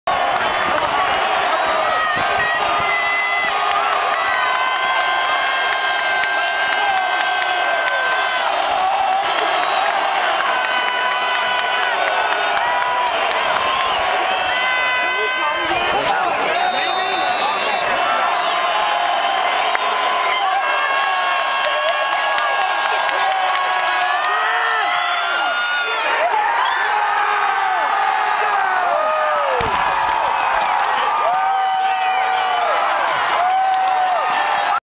Ewu vs. Montana Game Winning Field Goal
Go ahead field goal with 9 seconds left in debut of red turf, 9/18/2010